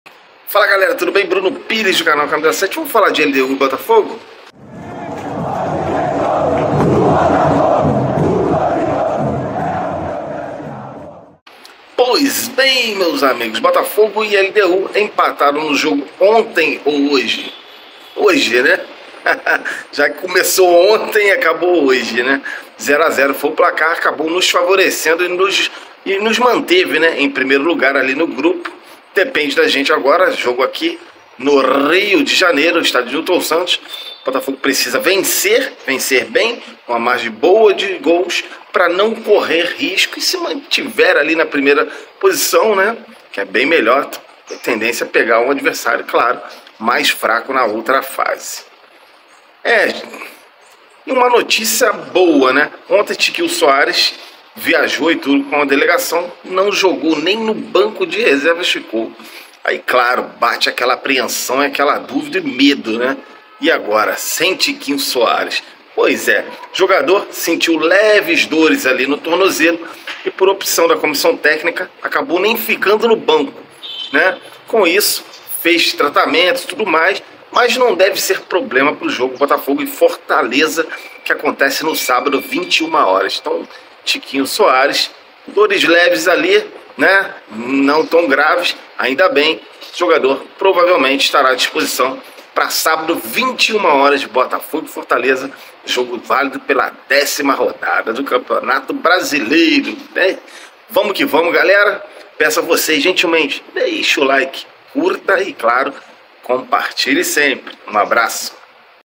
BOTAFOGO SE RECUPERA | TIQUINHO SOARES DEVE VOLTAR
LDU e Botafogo empataram pela Copa SULAMERICANA

Canal CAMISA7

Estamos também nas plataformas:

TikTok | Instagram | Kwai | Facebook